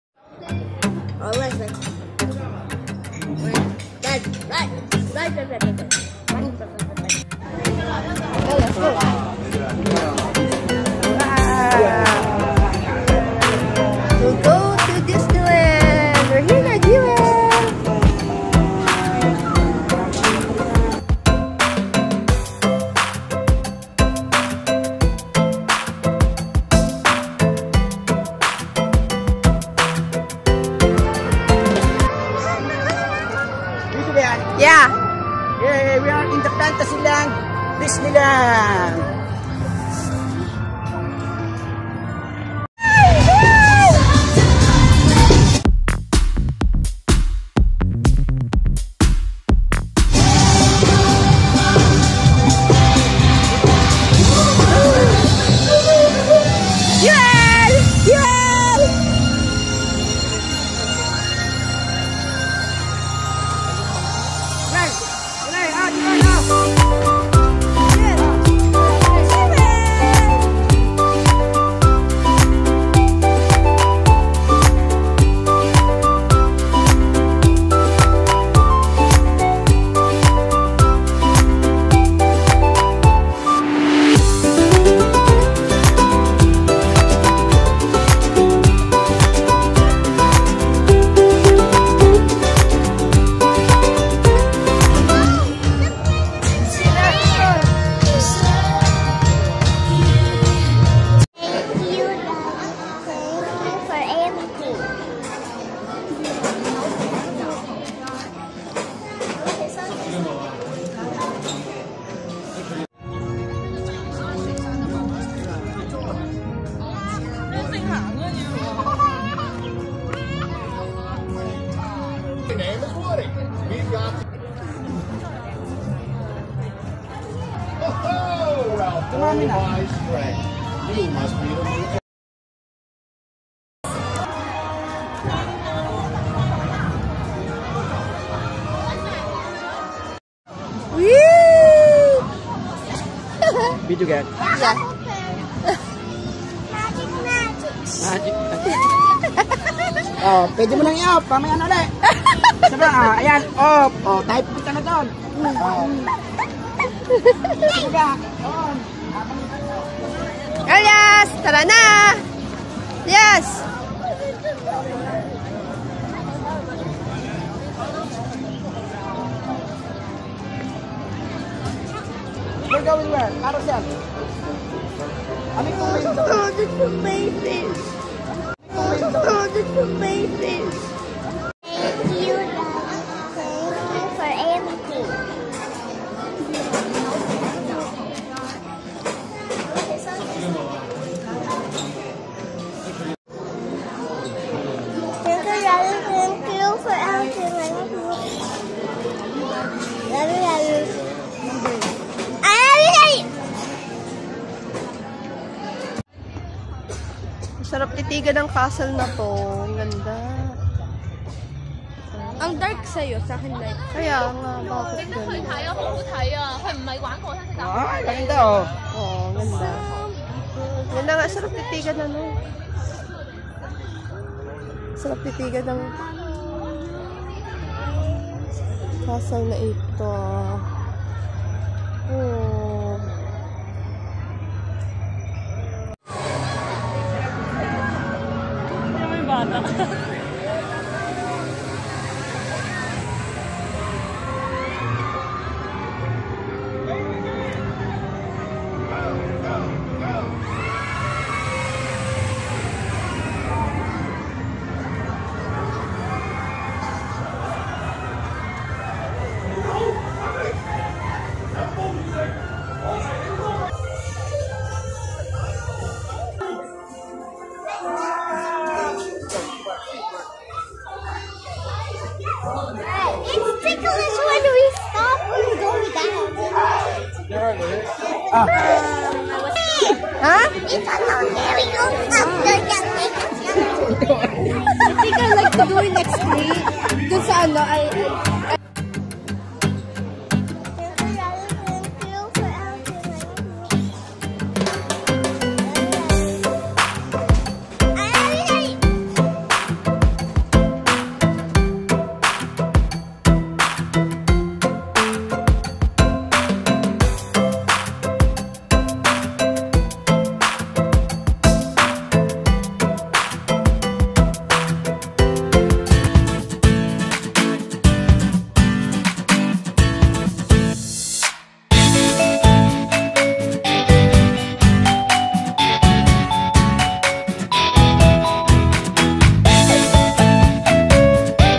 Disneyland Hong Kong ( Part 1 )
Fun family and memorable Hongkong Disneyland adventure. Part 1.